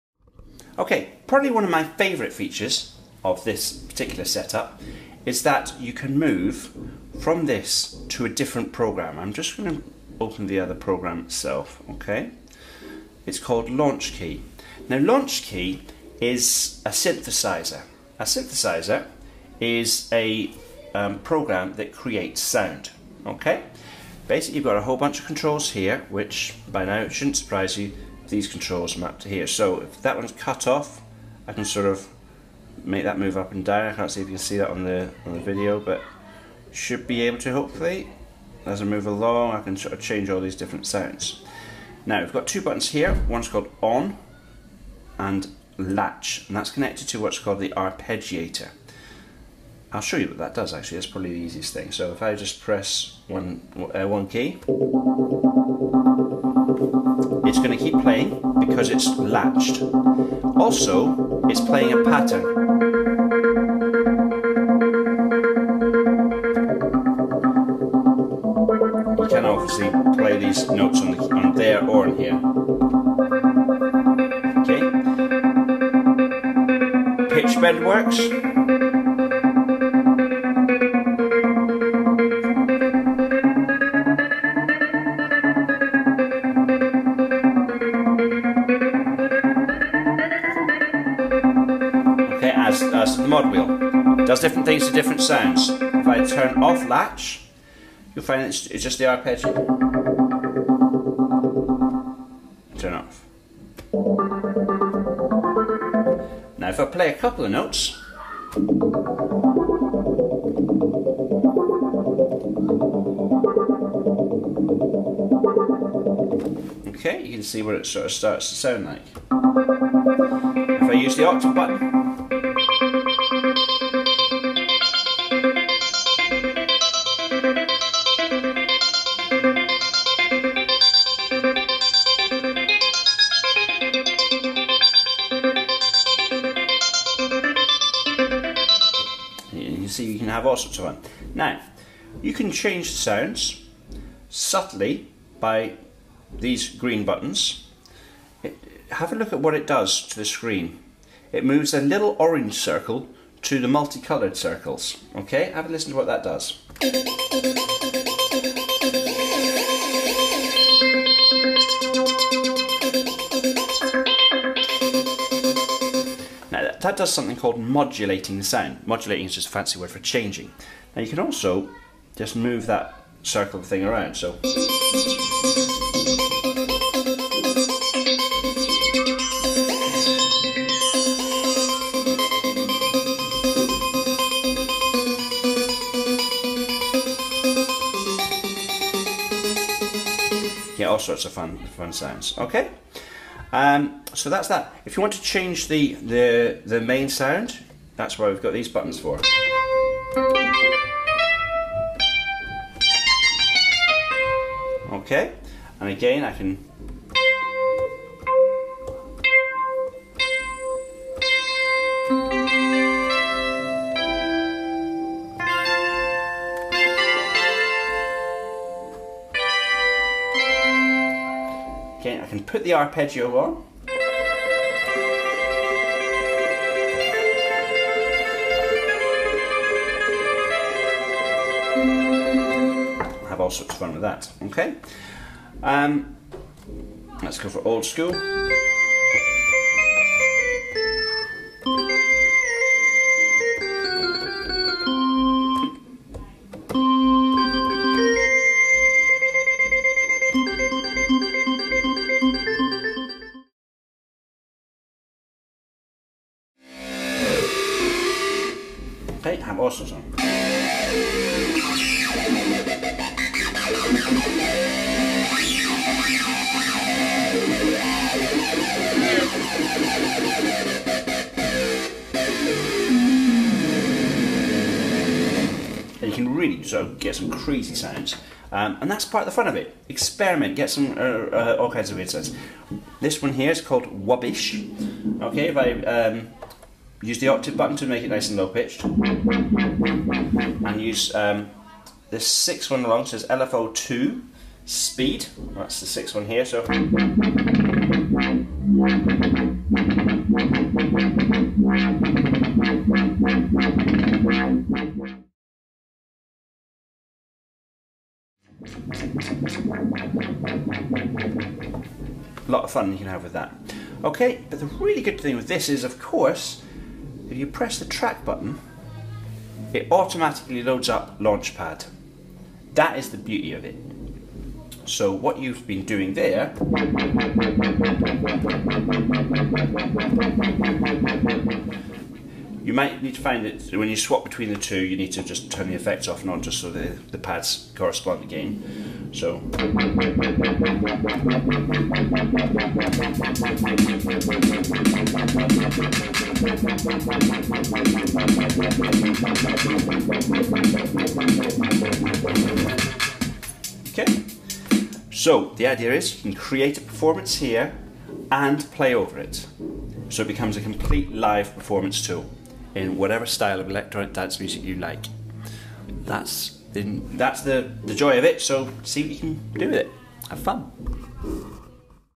Launch Project 5 Launchkey App
An introduction to using the Novation Launchkey App with the Launchkey keyboard controller and Launchpad App.